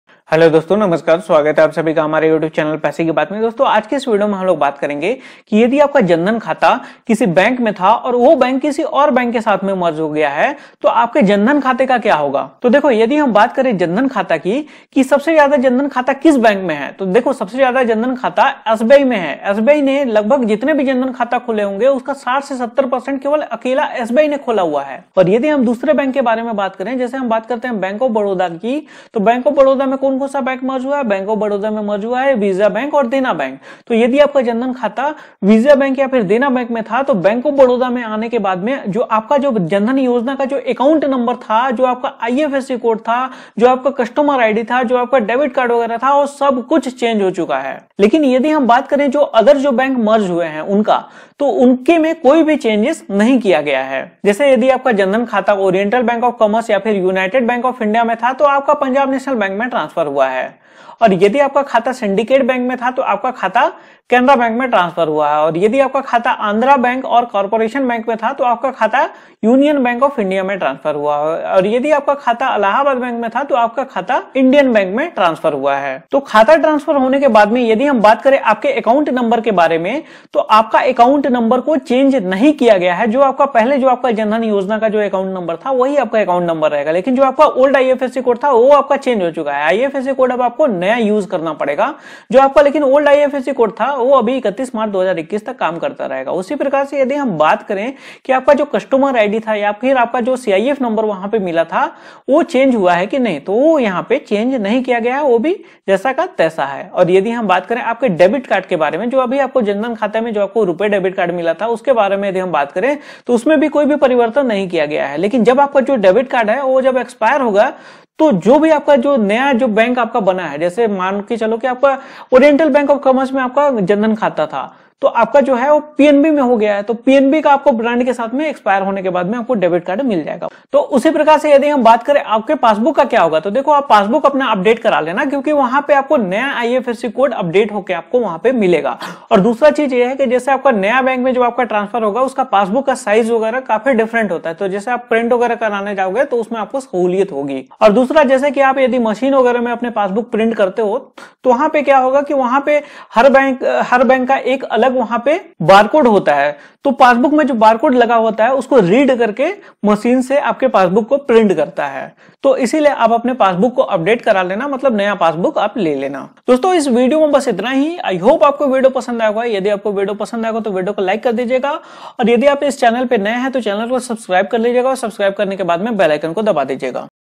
Bank Merger Ke Baad Jan Dhan Account Ka Kya Hoga?
In this video, you learn about, Bank Merger Ke Baad Jan Dhan Account Ka Kya Hoga? | PMJDY Account Bank Merger